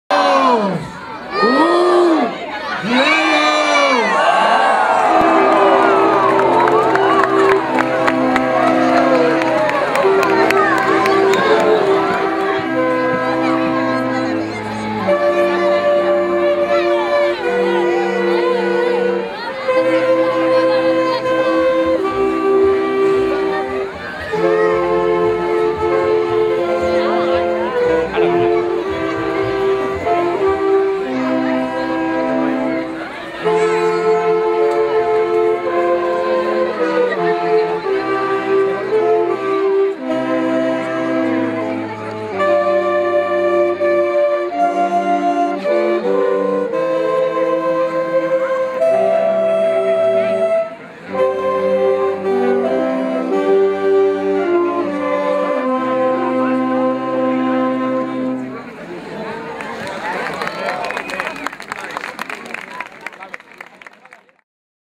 Encesa de l'arbre de Nadal a Valldoreix
Les llums de nadal ja brillen a Valldoreix!